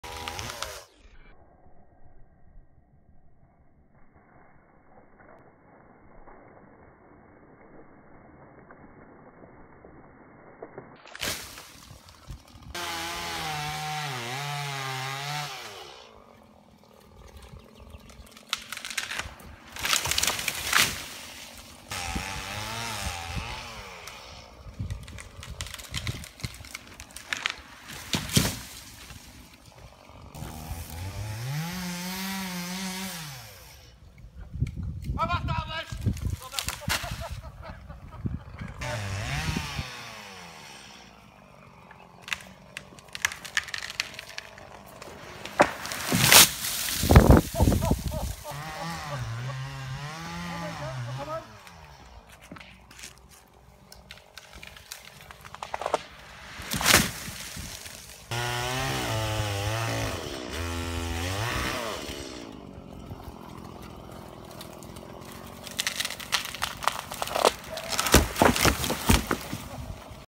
Felling av 6 trær på 70 sekunder (6 Tree cuttings in 70 seconds)
via YouTube Capture:
... Well actually this short film is 70 seconds, while the time spent in the forest to cut the 6 trees, remove branches and cut the trees into 10 foot logs took 90 minutes for me and my brother.